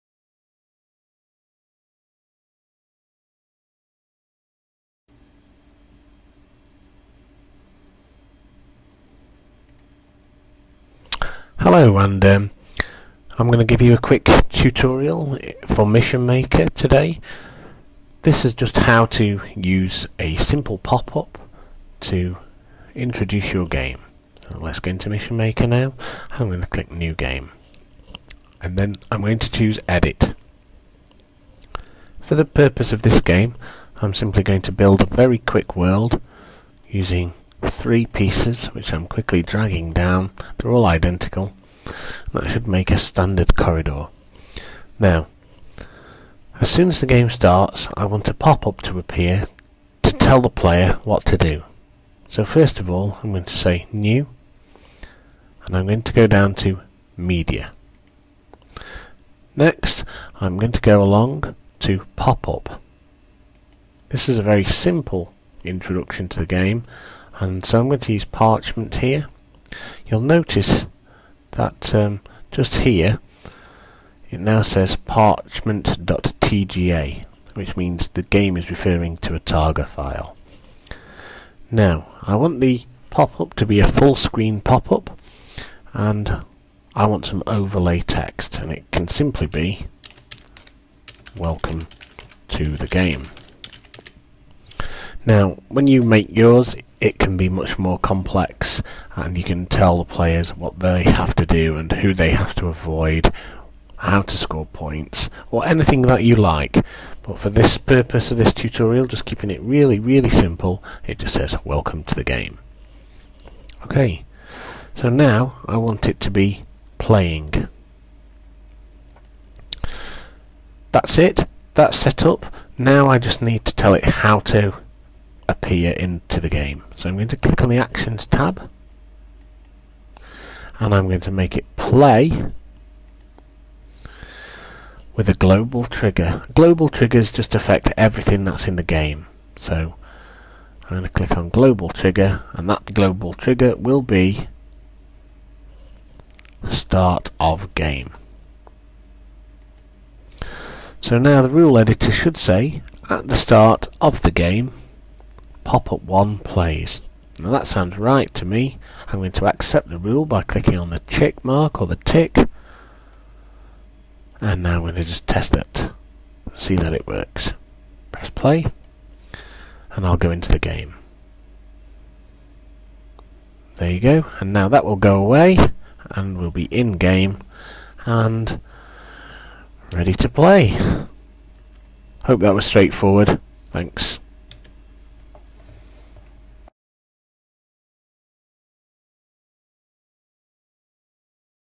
Mission Maker - Simple Popups
Create a Welcome screen for your game by Immersive Education Limited